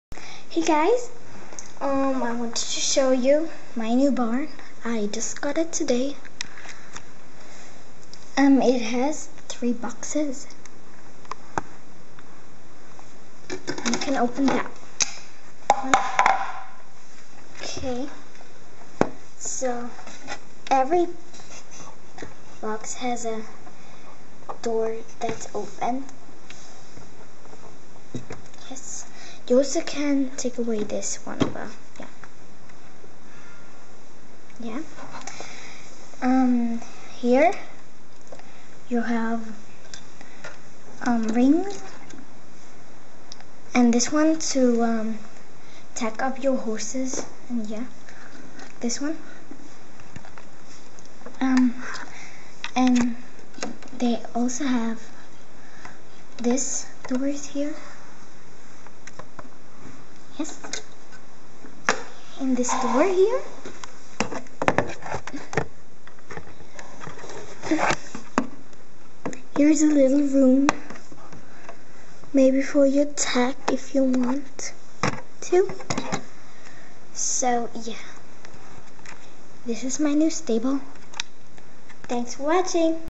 My new Schleich Stable!!!
I just bought it today yay im sooo happy about it! lol